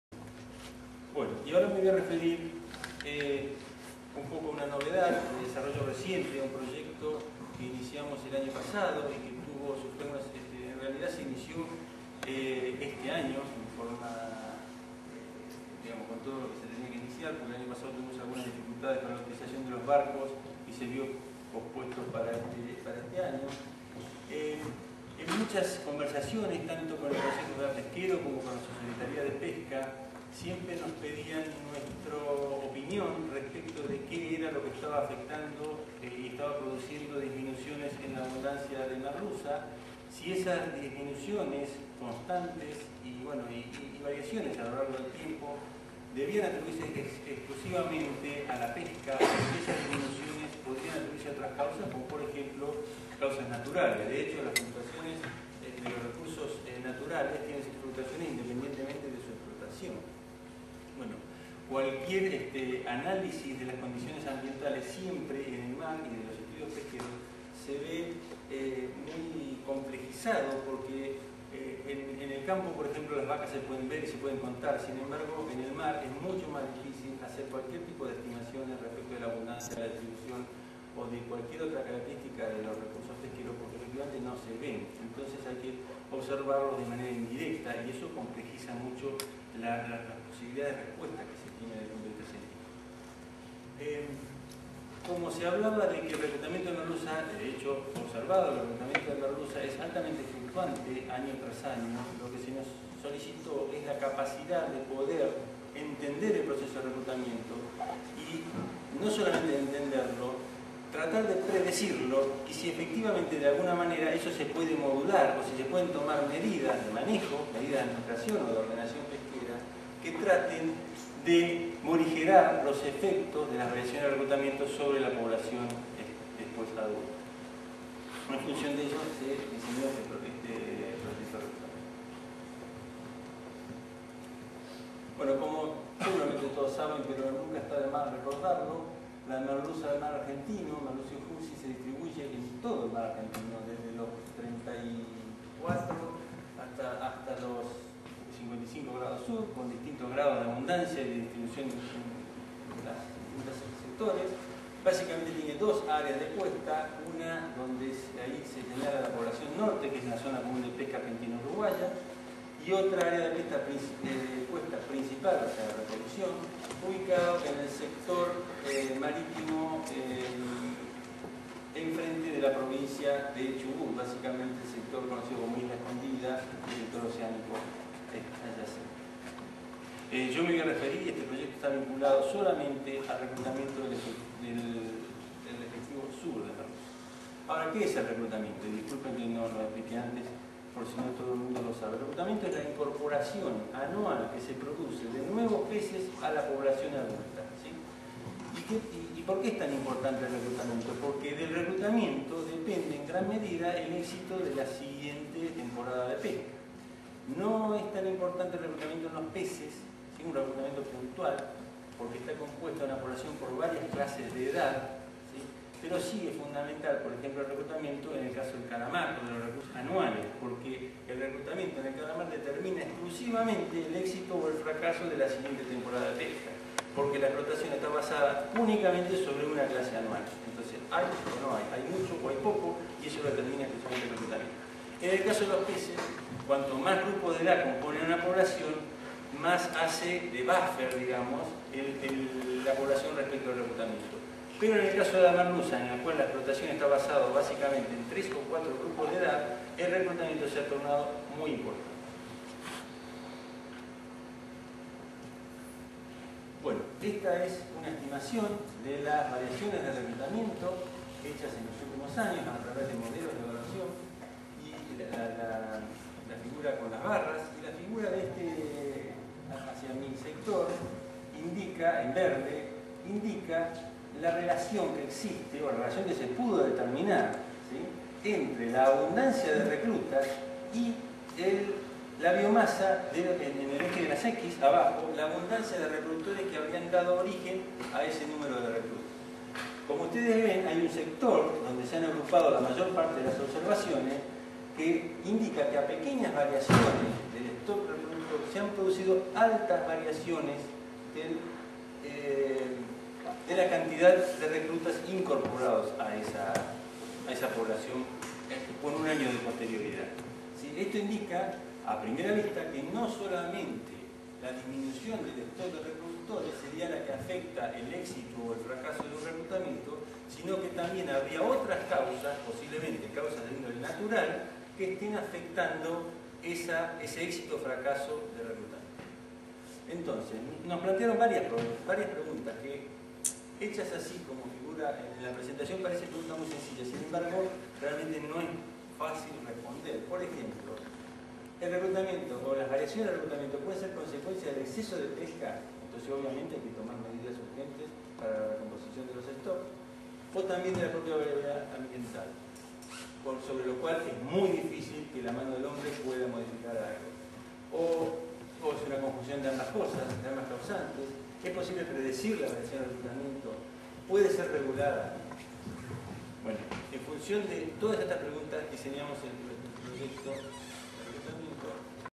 Dr. Otto Wöhler - Parte 2
Segunda parte de la exposición del Dr. Otto Wöhler, Director Nacional de Investigación del INIDEP, durante el primer panel de la Segunda Jornada 2009 del Ciclo de Conferencias sobre los Intereses Marítimos y la Defensa Nacional "Almirante Storni".

Organizada por el Centro de Estudios Estratégicos para la Defensa "Manuel Belgrano", esta Segunda Jornada fue transmitida mediante videoconferencia en la Escuela Nacional de Pesca (Mar del Plata), en el Auditorio San Martín del Ministerio de Defensa (Buenos Aires) y en la Universidad Nacional de la Patagonia "San Juan Bosco" (Comodoro Rivadavia).

Tema de la Segunda Jornada 2009: "Recursos Naturales del Mar Argentino: Desafíos y Perspectivas".

Segundo Panel: "Descripción del Escenario, Desafíos y Perspectivas".